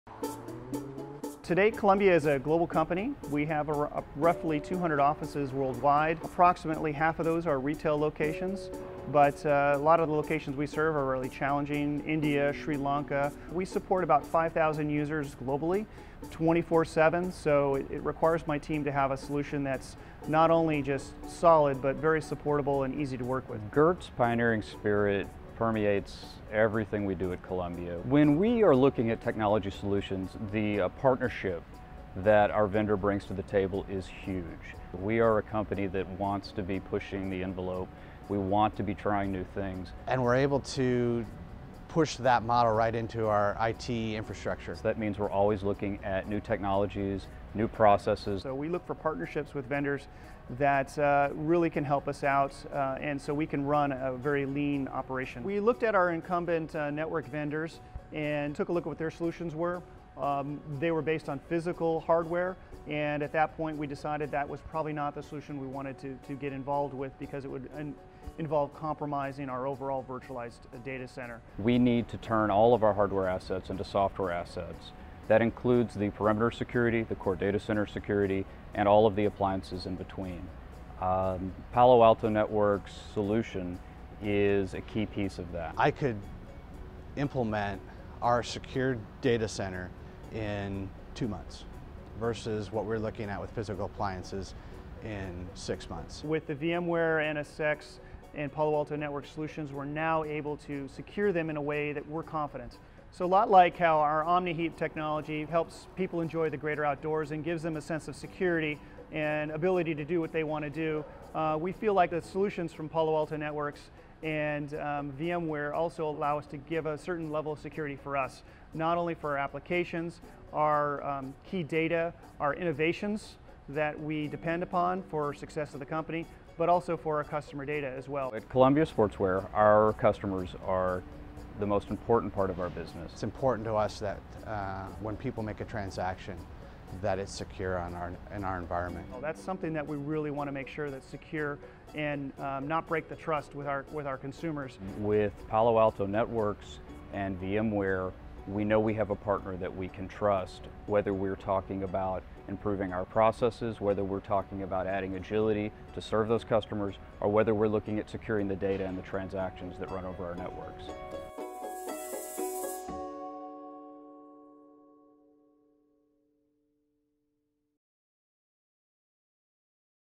Customer Spotlight: Columbia Sportswear
Columbia Sportswear is a global company that designs and delivers high-value outdoor products for active consumers of all ages. In this video, Columbia Sportswear's John Spiegel, Global IT Communications Manager, and Tim Melvin, Global IT Director, talk about embracing the Palo Alto Networks/VMware NSX virtualized data center platform, and how the platform helps Columbia Sportswear achieve top-notch data center security without slowing business productivity.